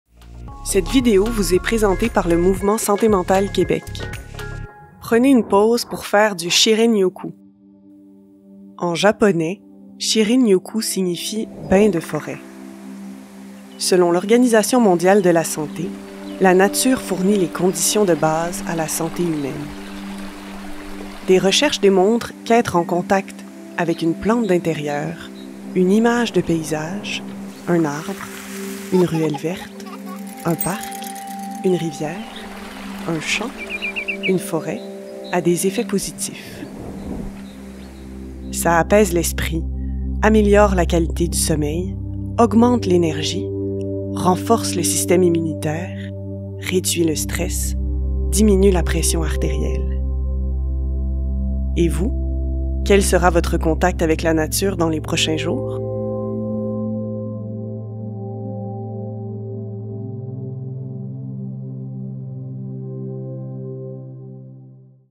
Prenez une pause pour faire du shinrin-yoku
En japonais, shinrin-yoku signifie « bain de foret ». Quel sera votre contact avec la nature dans les prochains jours?